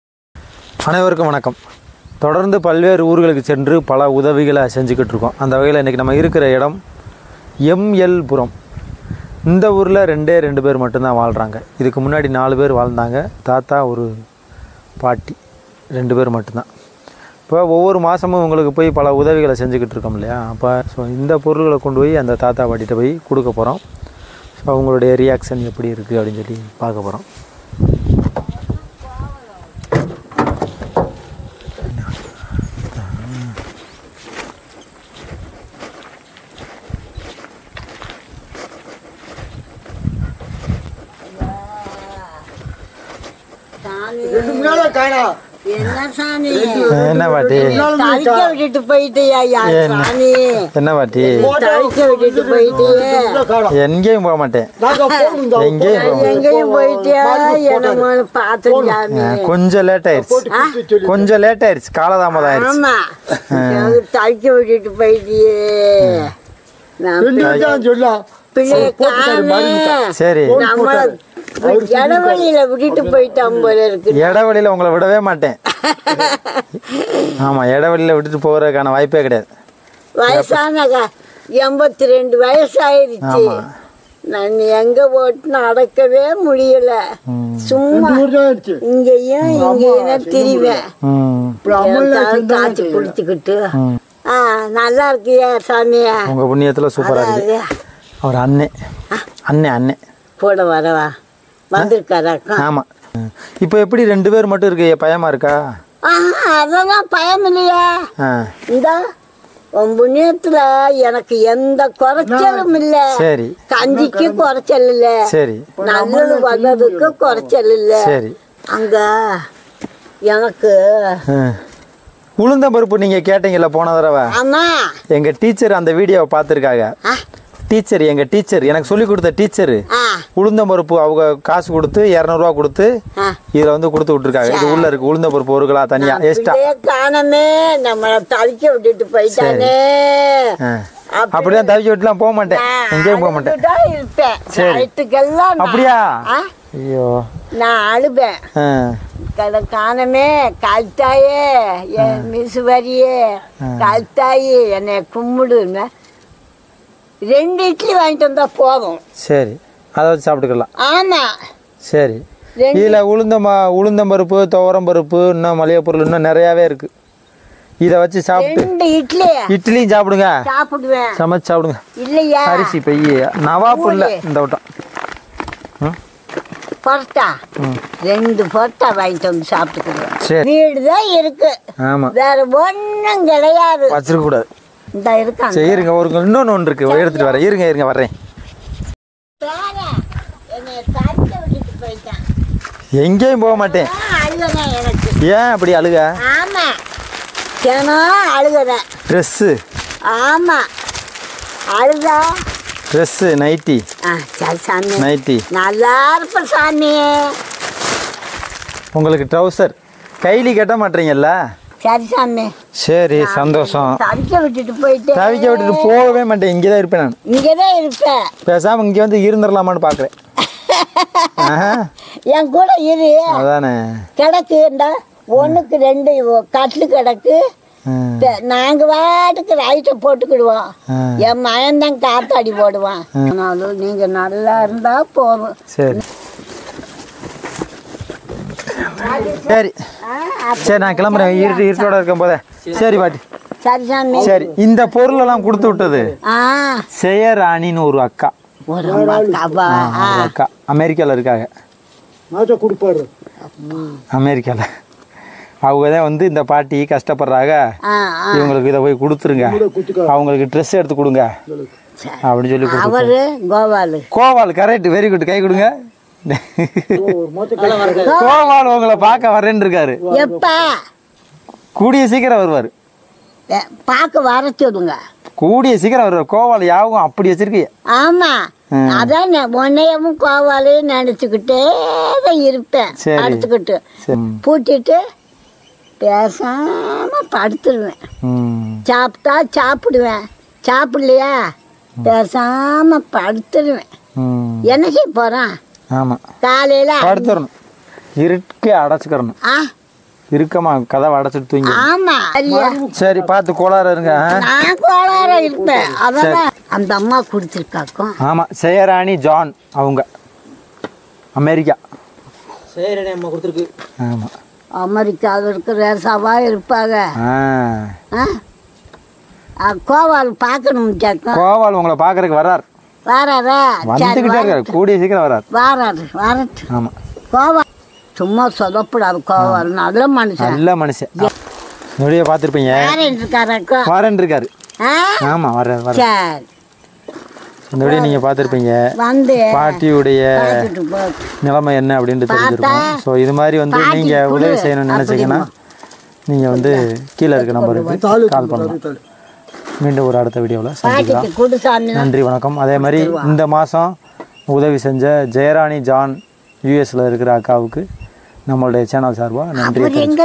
இரண்டு பேர் மட்டுமே அதிசய வாழும் கிராமம் முதுமையில் தனிமை மிகவும் கொடியது
இரண்டு பேர் மட்டுமே வாழும் கிராமம்